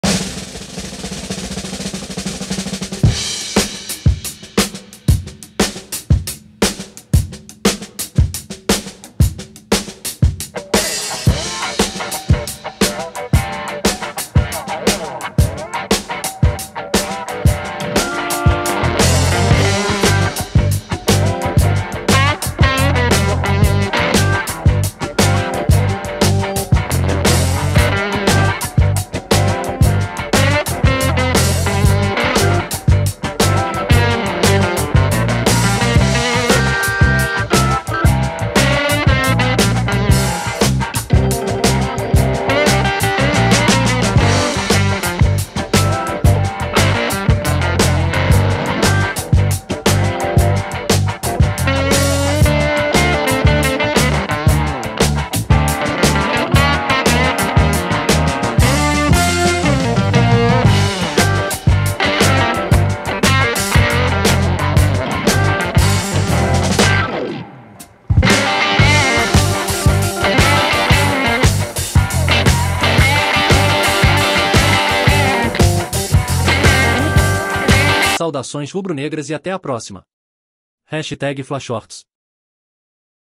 💪🔴⚫ ATHLÉTICO-PR 2 a 1 FLAMENGO: GOLS e MELHORES MOMENTOS
Sejam bem-vindos ao canal FLA Shorts, um canal de vídeos curtos com notícias, curiosidades e tudo sobre o

Confira tudo sobre a partida entre Athletico-PR e Flamengo, com destaque para os gols marcados e os melhores momentos do jogo